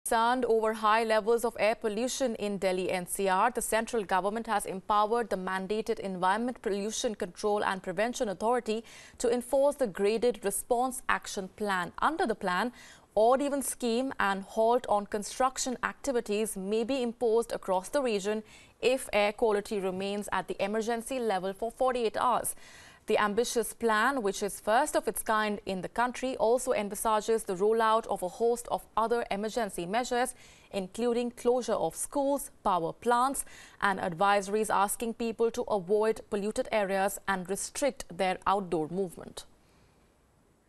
Delhi gets new pollution action plan
Concerned over high levels of air pollution in Delhi-NCR, the Central government has empowered the mandated Environment Pollution Control and Prevention Authority to enforce the graded response action plan. Here's more to this story